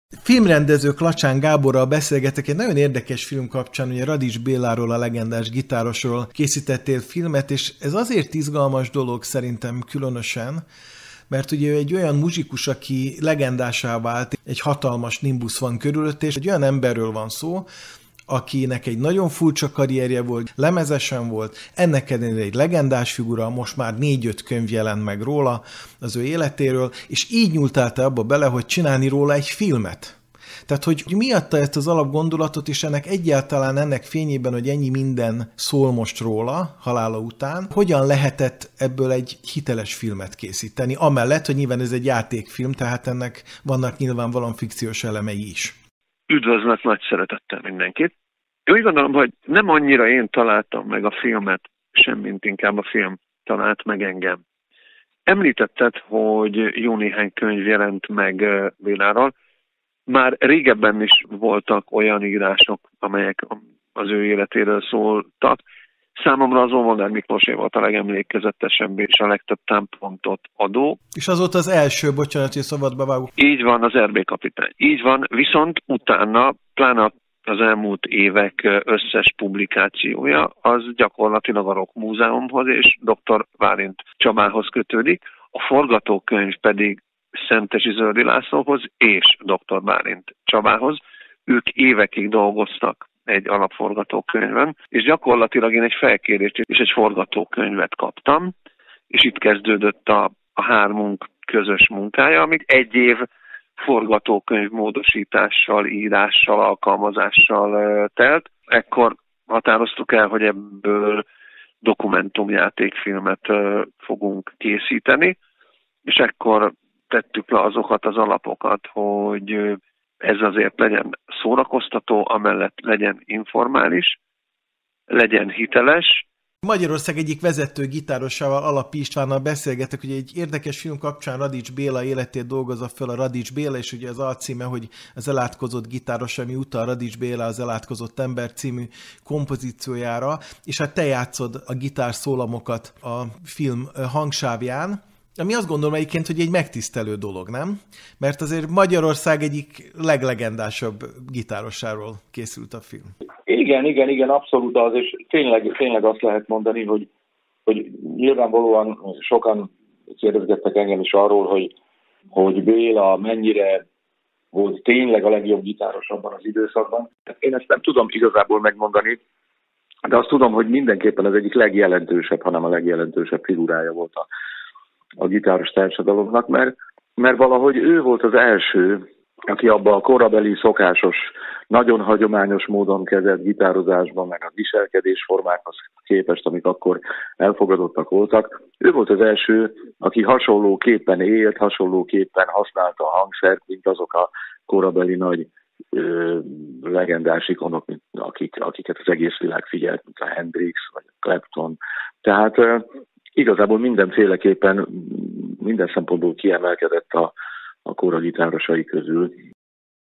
Interjú Klacsán Gáborral és Alapi Istvánnal a Radics Béla filmről részlet
Interjúk az új "Radics Béla - Az elátkozott gitáros" című filmről.